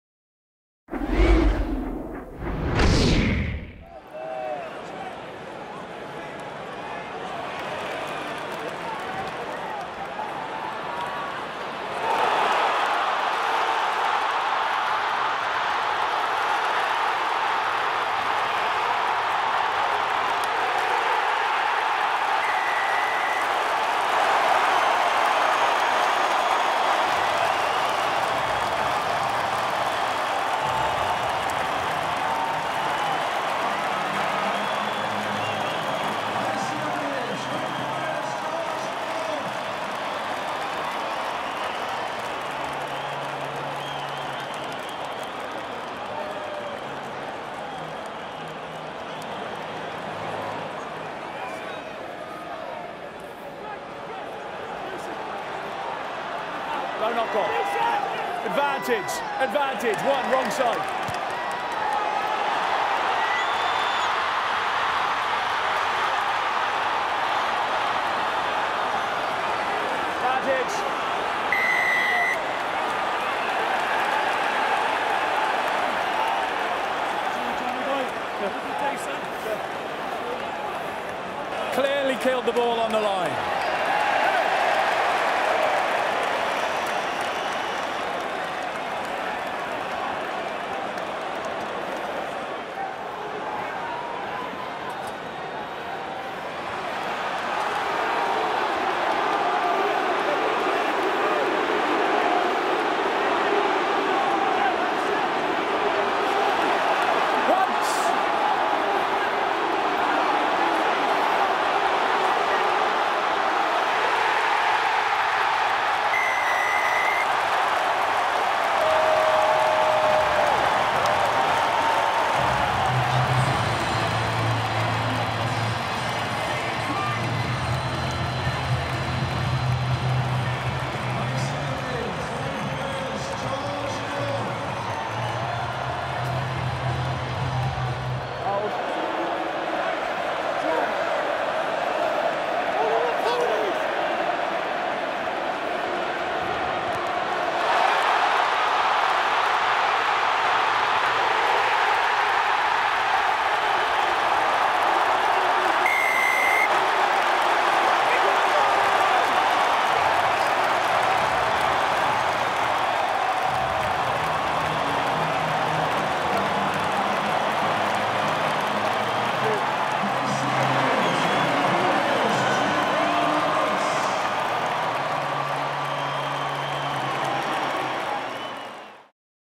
Short Highlights: Wales 22-9 Ireland | RBS 6 Nations
Check out the short highlights of Wales v Ireland at Principality Stadium.

Wales v Ireland | 10 March 2017 | Principality Stadium

Wales: 15 Leigh Halfpenny, 14 George North, 13 Jonathan Davies, 12 Scott Williams, 11 Liam Williams, 10 Dan Biggar, 9 Rhys Webb, 8 Ross Moriarty, 7 Justin Tipuric, 6 Sam Warburton, 5 Alun Wyn Jones (c), 4 Jake Ball, 3 Tomas Francis, 2 Ken Owens, 1 Rob Evans

Replacements: 16 Scott Baldwin, 17 Nicky Smith, 18 Samson Lee, 19 Luke Charteris, 20 Taulupe Faletau, 21 Gareth Davies, 22 Sam Davies, 23 Jamie Roberts

Ireland: 15 Rob Kearney, 14 Keith Earls, 13 Garry Ringrose, 12 Robbie Henshaw, 11 Simon Zebo, 10 Johnny Sexton, 9 Conor Murray, 8 Jamie Heaslip, 7 Sean O’Brien, 6 CJ Stander, 5 Devin Toner, 4 Donnacha Ryan, 3 Tadhg Furlong, 2 Rory Best (c), 1 Jack McGrath

Replacements: 16 Niall Scannell, 17 Cian Healy, 18 John Ryan, 19 Iain Henderson, 20 Peter O’Mahony, 21 Kieran Marmion, 22 Paddy Jackson, 23 Tommy Bowe